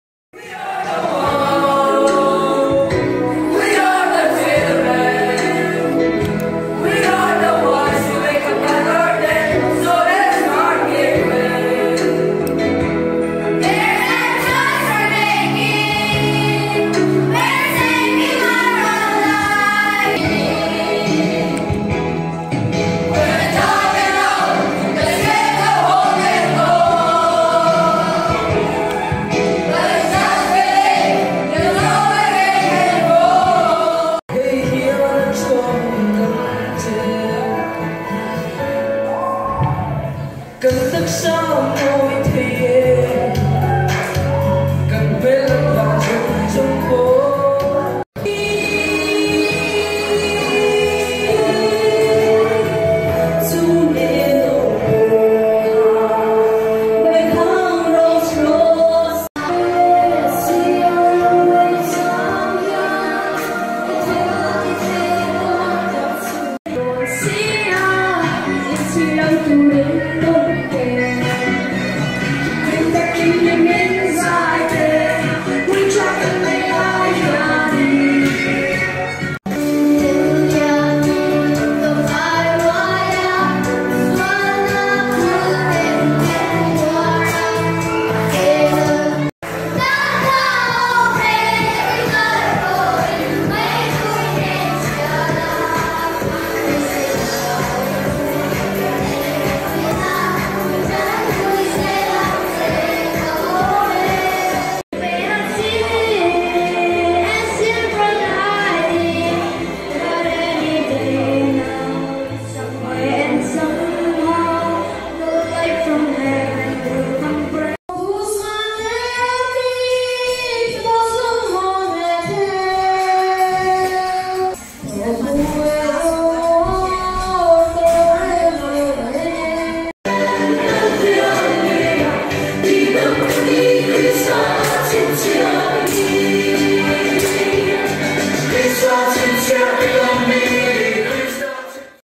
Rehearsal Govt Venglai MS Students
Kan Students ten Kan Headmaster Pu C Laltlansanga Pension thlah nan a, an inbuatsaihna thenkhat Video clip tawi te te a dah khawm ani e..

30th Oct 2019